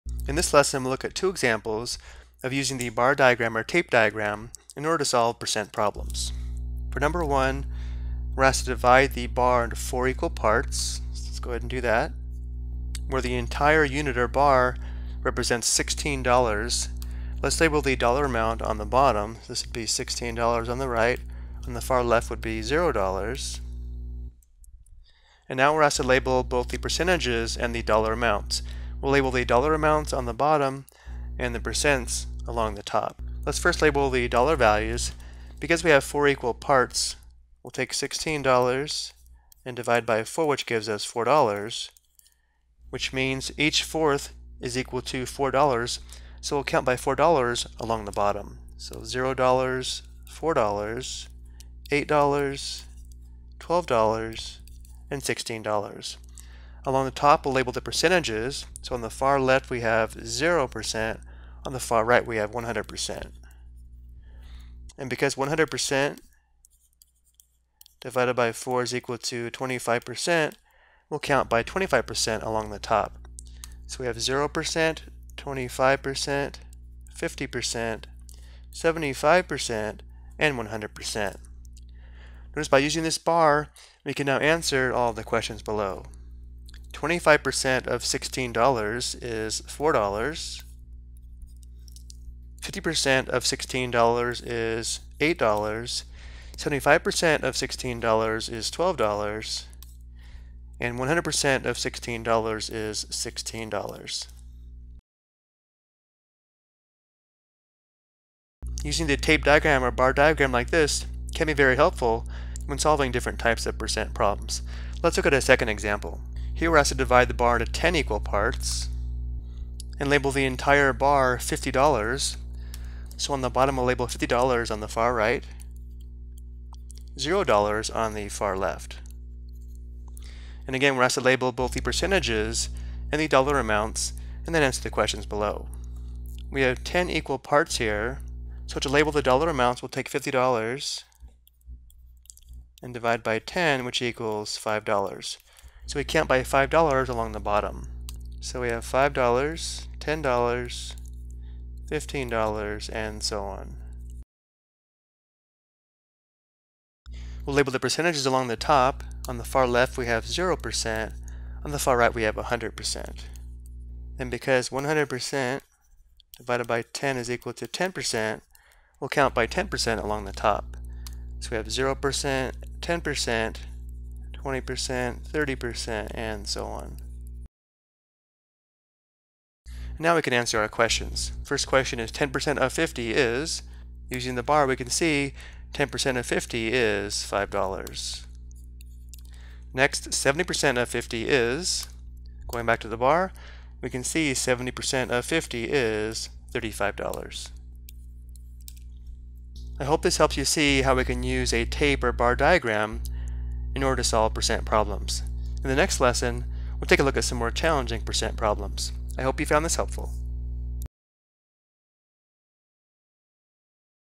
Solve Basic Percent Problems Using a Tape Diagram (Bar Diagram)
This video explains how to solve percent problems using a tape diagram or bar diagram.